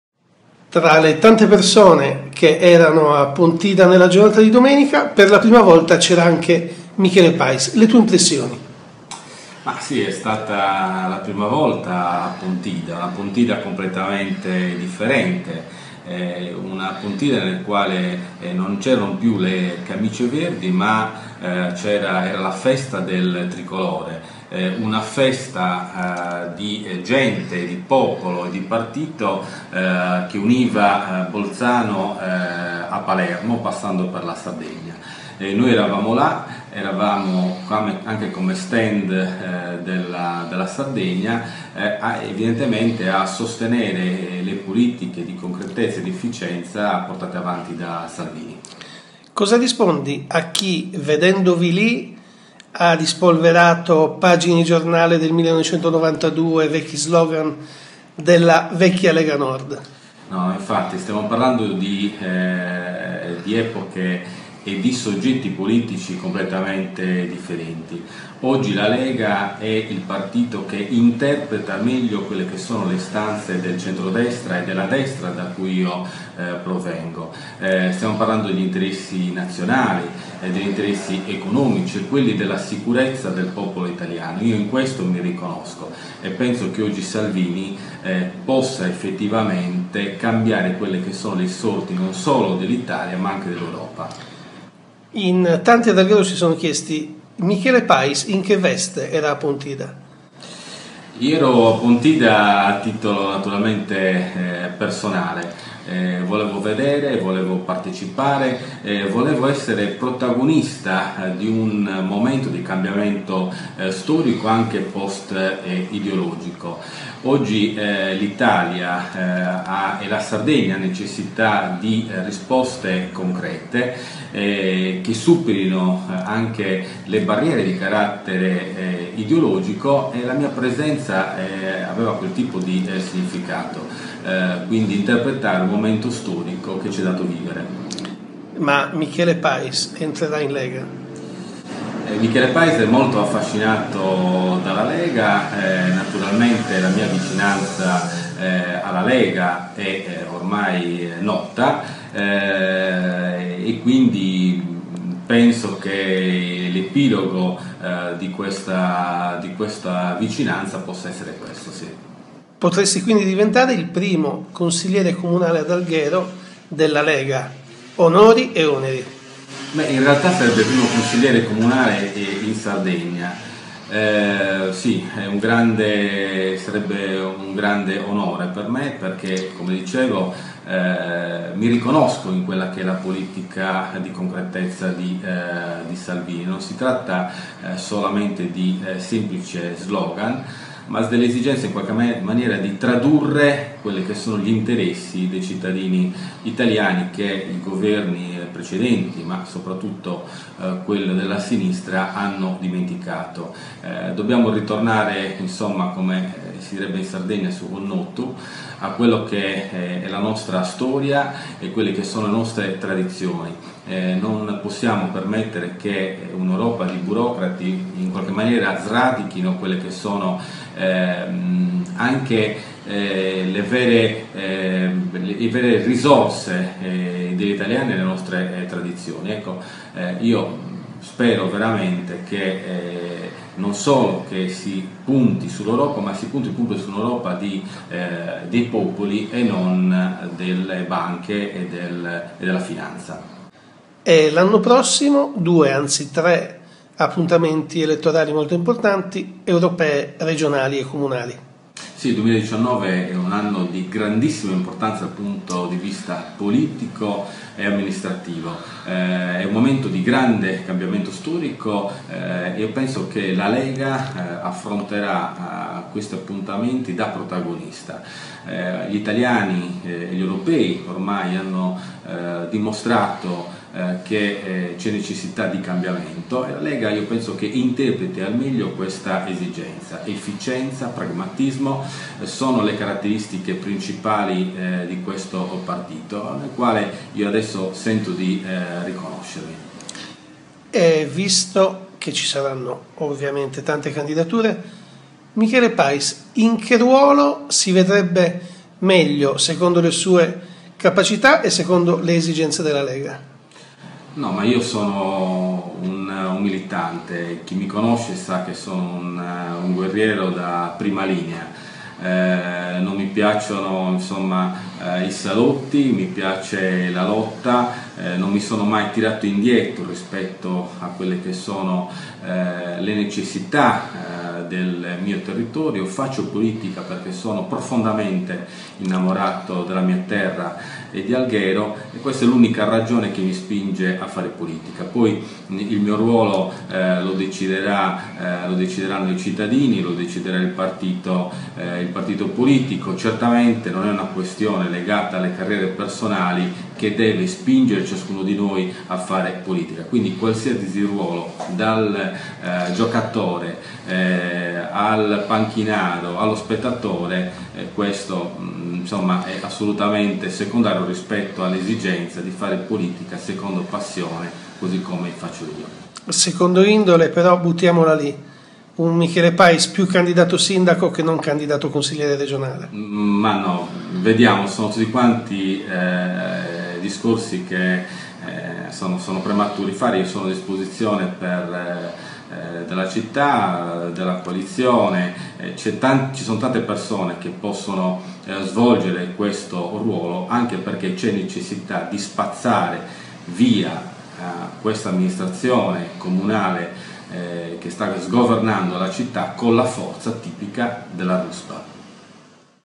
Michele Pais in Lega, dichiarazione d’impegno
Michele Pais abbandona definitivamente i berlusconiani Camerada e Pirisi. Si dice pronto a mettersi in gioco per il bene del territorio, ma la sua lunga esperienza politica, il peso elettorale e i trascorsi non potranno non essere considerati dagli alleati. E´ una vera e propria dichiarazione d´impegno quella che rivolge pubblicamente nei confronti della Lega e di Matteo Salvini. «C´è bisogno di ritornare immediatamente a Su Connottu. Non mi piacciono i salotti, sono un combattente di prima linea che non si è mai tirato indietro rispetto a quelle che sono le necessità dei cittadini». Il leader dell´opposizione in Consiglio ad Alghero si mette definitivamente alle spalle l´esperienza con Forza Italia e i Teddiani e rilancia: «Faccio politica perchè sono profondamente innamorato della mia terra e di Alghero. Il mio ruolo lo decideranno i cittadini e il partito politico». La sua intervista sul Quotidiano di Alghero.